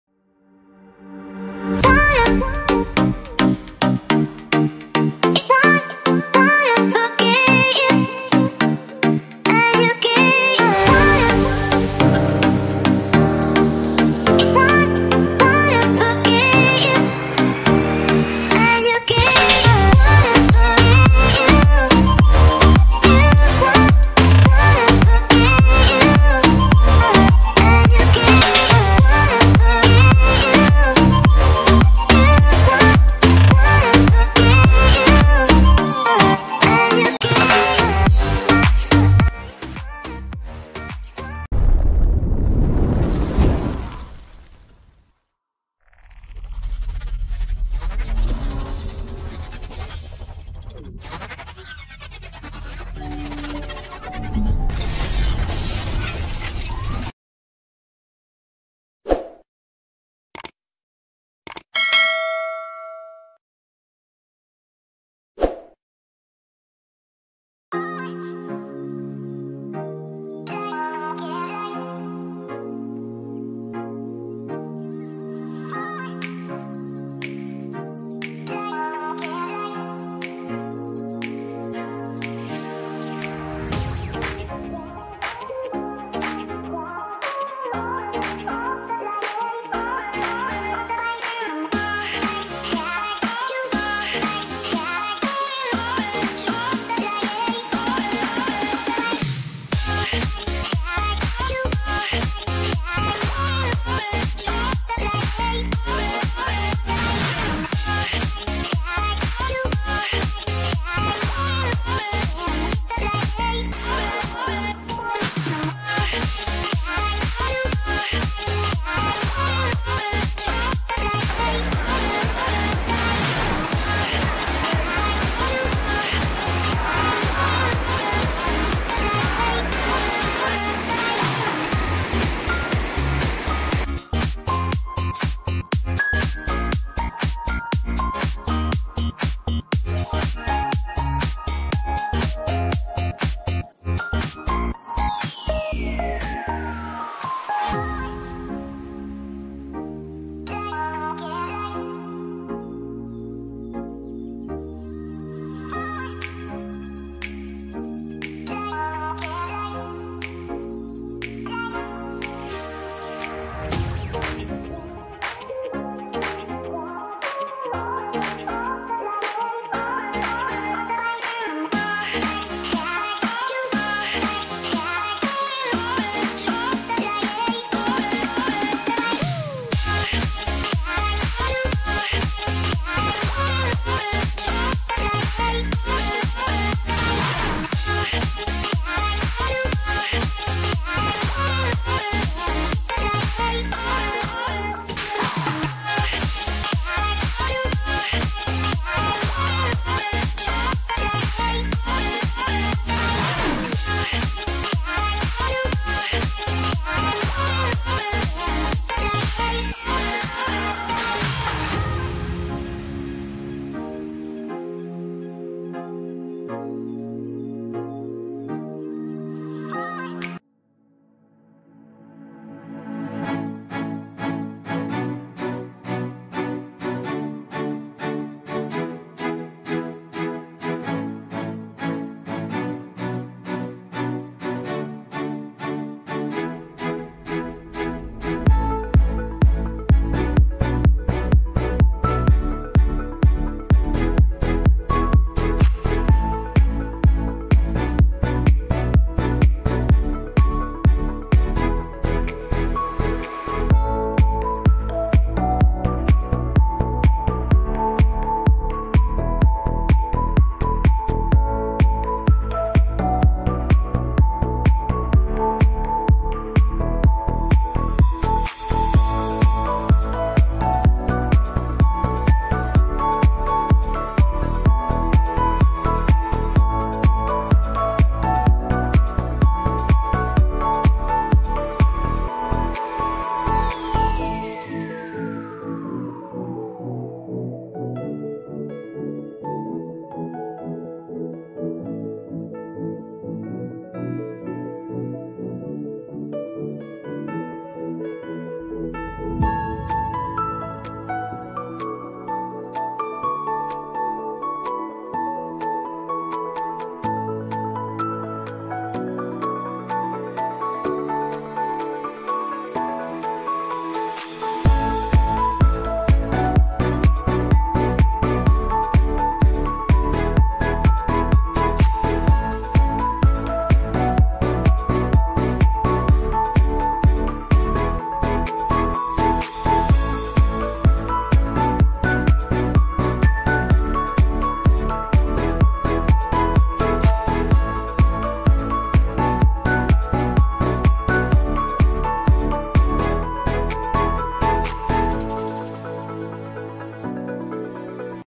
Pemandian alam Maripro sembahe
Pemandian alam maripro terletak di sembahe ,medan-sumatera utara.
Tempat wisata ini sangat bagus dan bersih sekali untuk destinasi pemandian alam yang masih natural .jadi anda tidak akan menyesal datang ketempat ini.

Pemusik: @iksonmusic